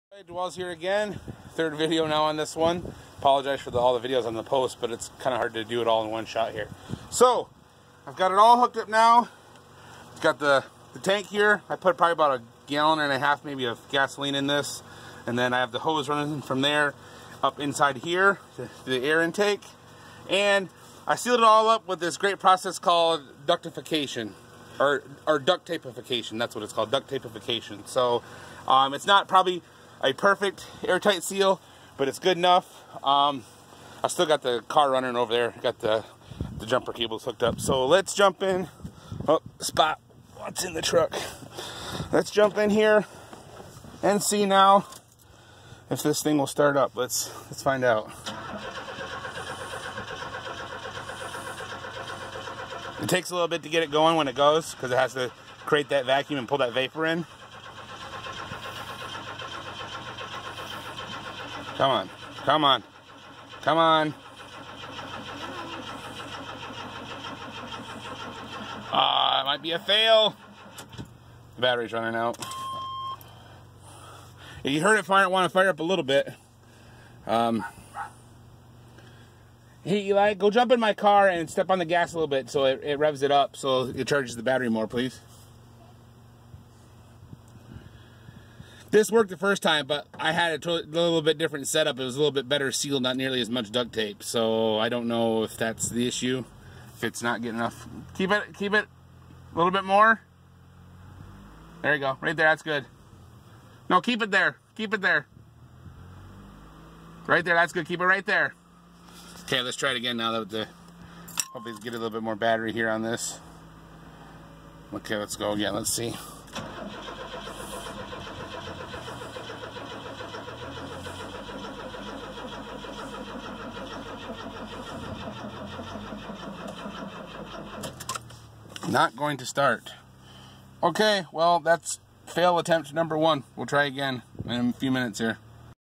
1991 Chevy S10 Fuel Vaporizer first test. Failed.
Hooked a fuel vaporizer up to my 91 Chevy S10 and tried to get the truck to run on it and it failed the first time.